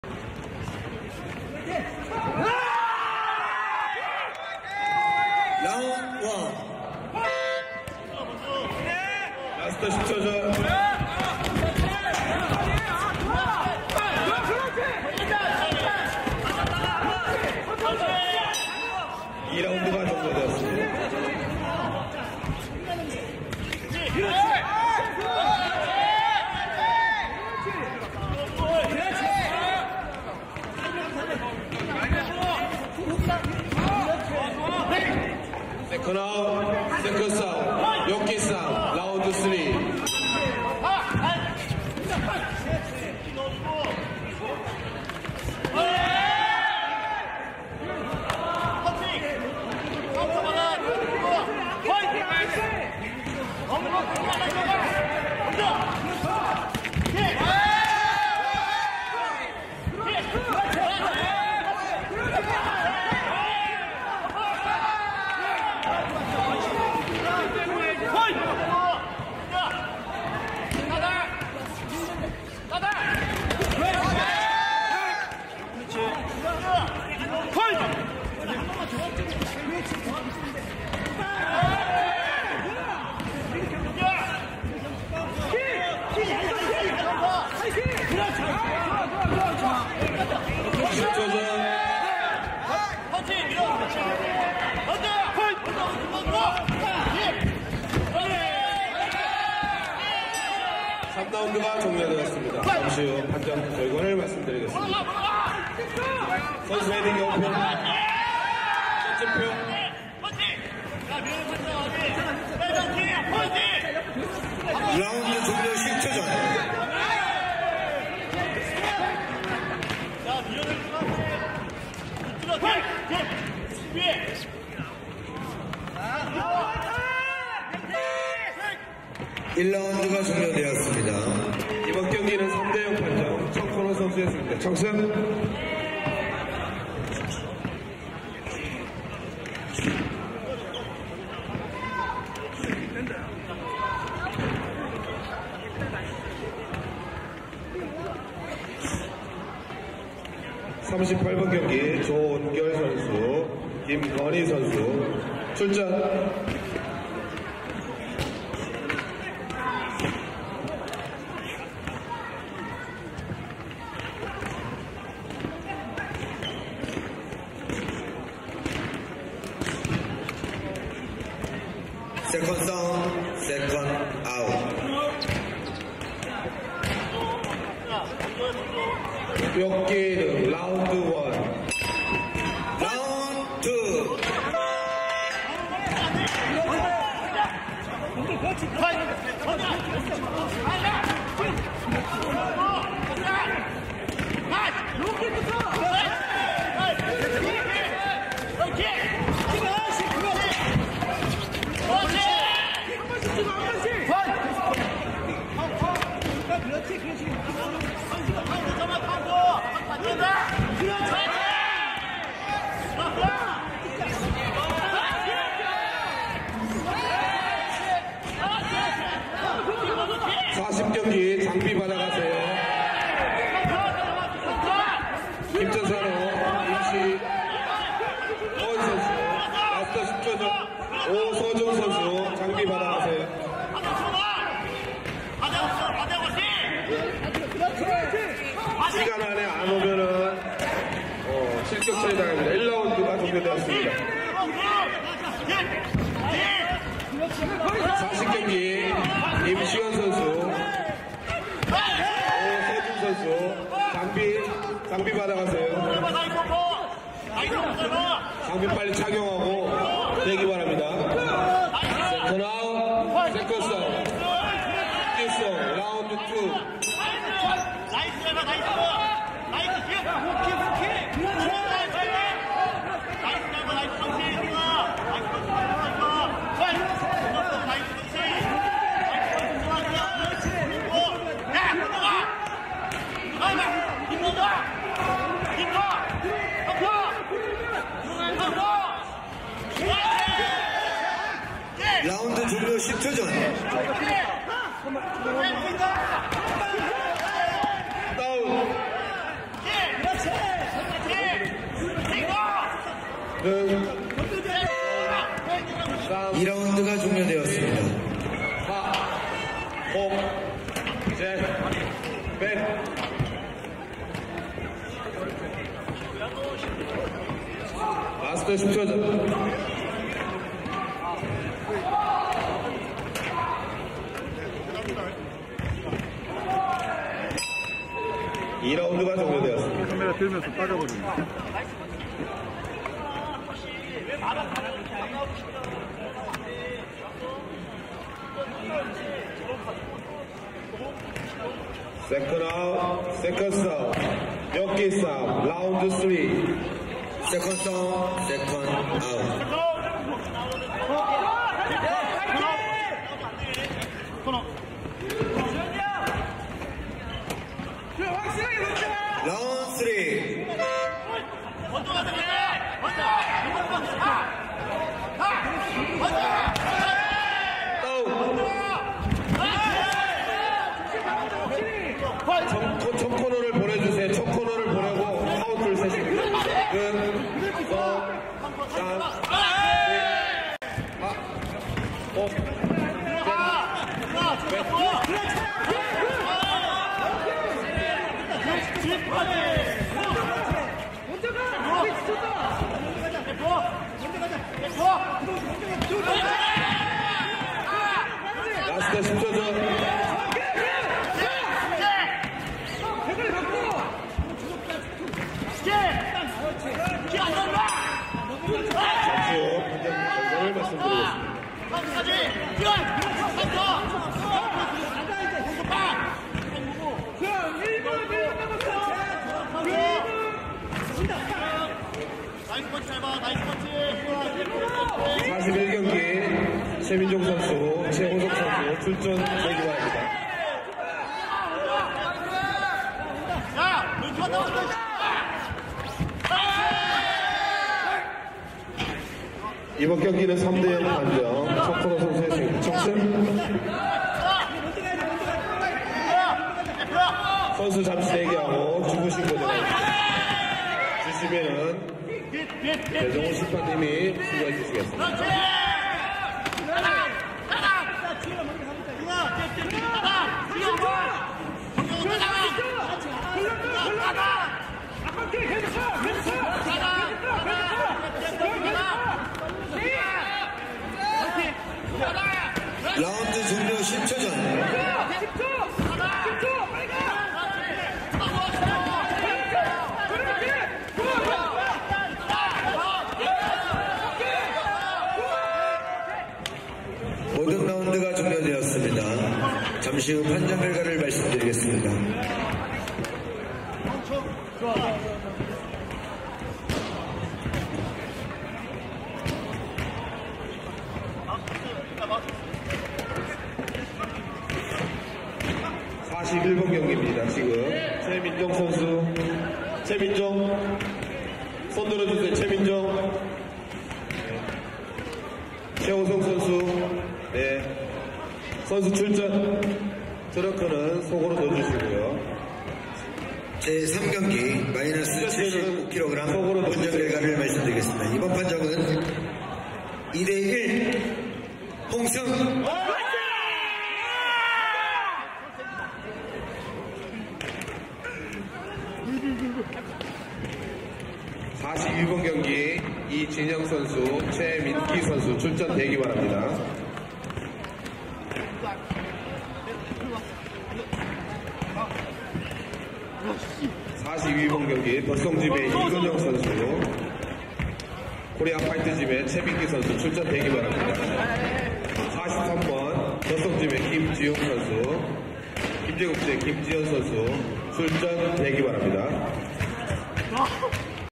MAX FC 퍼스트리그30-3경기-김주현(제주팀일도) VS 전승훈(청주오창투엑스짐)
대회명 : MAX FC 퍼스트리그 30
날짜 : 2024년 3월 10일(일) 1시
장소 : 익산 실내체육관

3경기 -75kg 김주현(20/제주팀일도) VS 전승훈(23/청주오창투엑스짐)